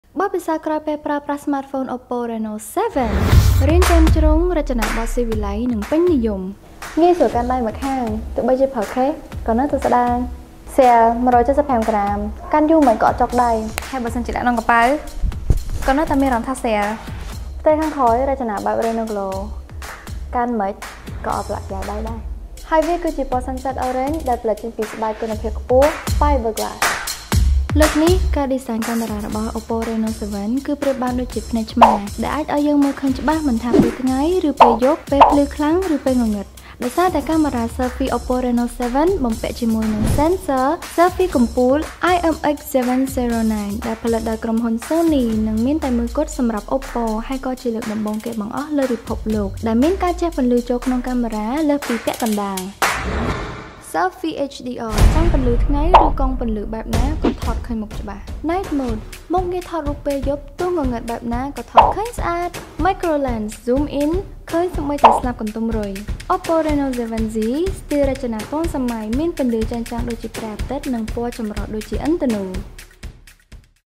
បើ Jennie VEE ក្លាយជាអ្នក Review ទូរស័ព្ទ
៌៌៌៌៌៌៌៌៌៌៌៌៌៌៌៌៌OPPO Reno7 មានតម្លៃ 369$
OPPO Reno7 Z 5G មានតម្លៃ $429